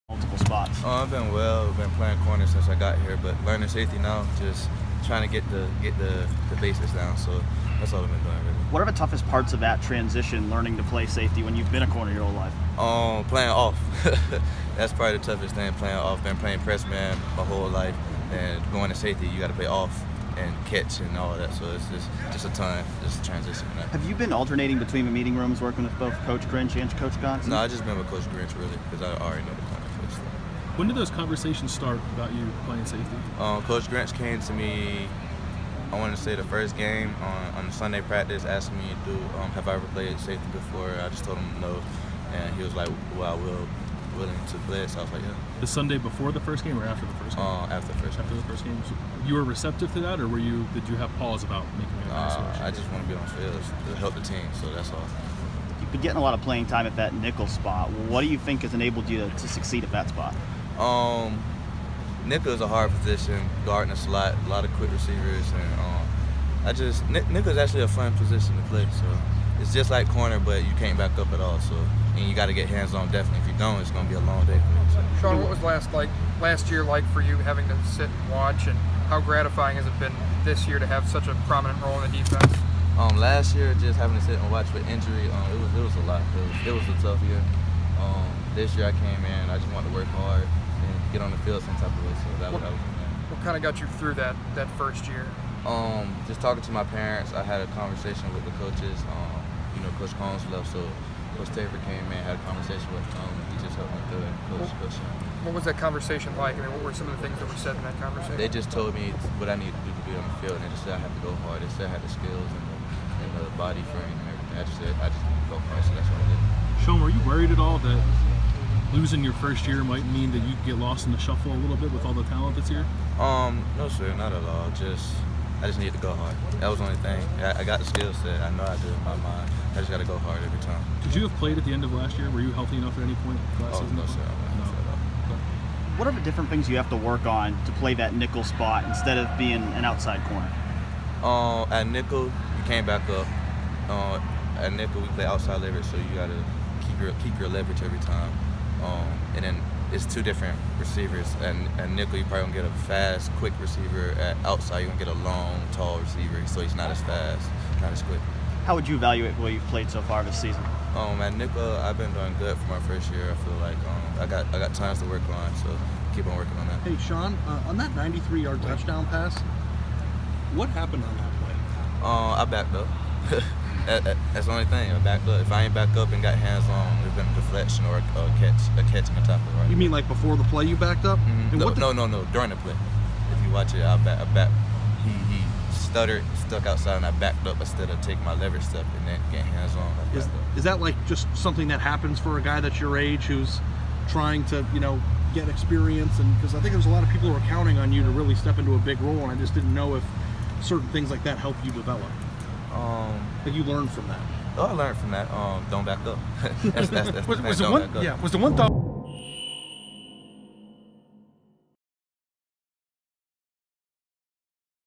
Shaun Wade healthy and pushing for Ohio State safety job
Wade battled through injury last year to become Ohio State’s starting nickel corner this year.